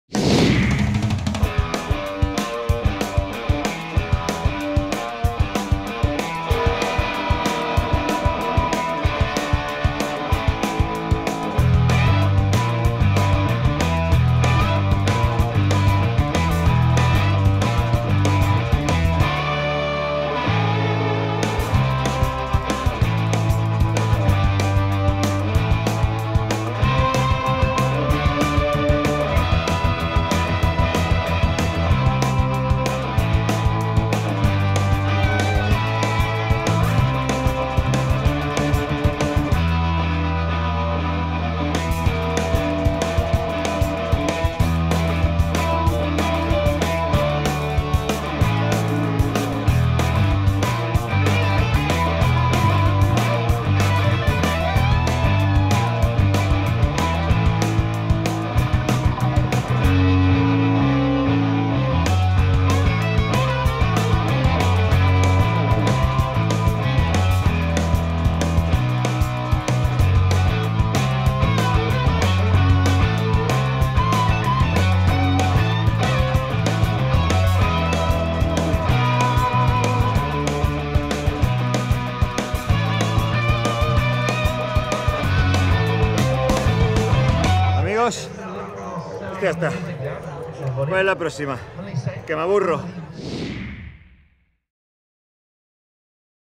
Protextrem acompaña a nuestro equipo Imparables en la Absa Cape Epic 2014
Protextrem no se ha querido perder la oportunidad de compartir con el equipo Imparables el nuevo desafío para este 2014: La Absa Cape Epic. Descubre cómo lo vivieron los corredores.